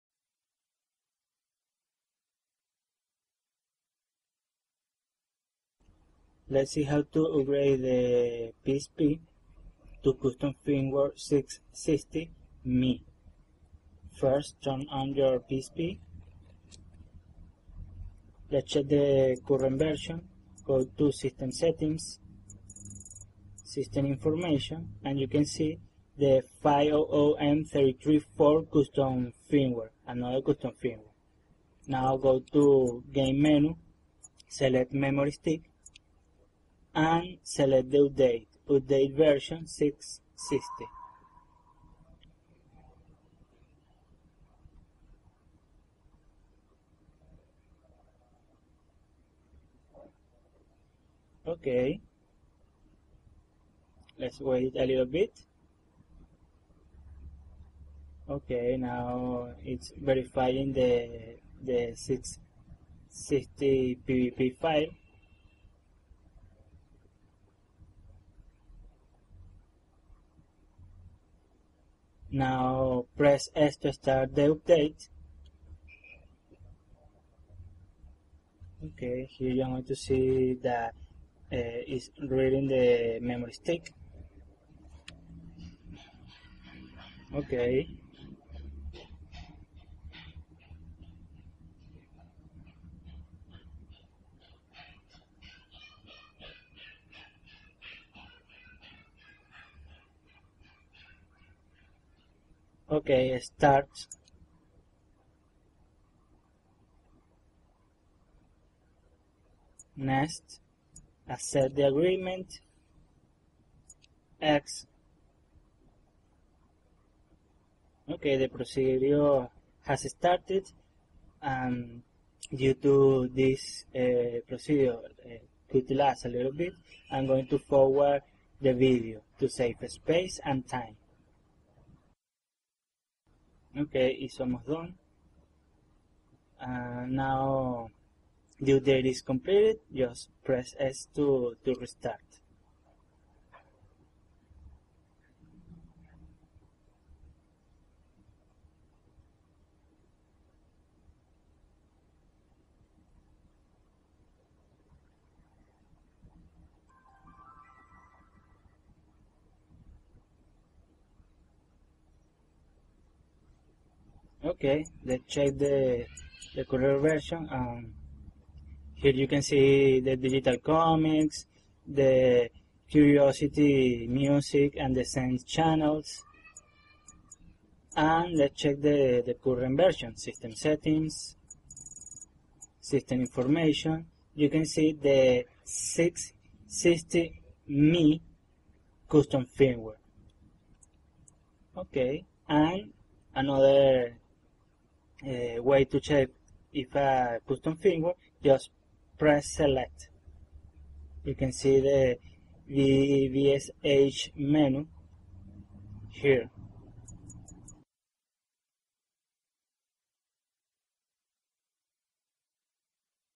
Installing Custom Firmware 6.60 ME
On this episode you are going to see how to install custom firmware 6.60 ME on the PSP.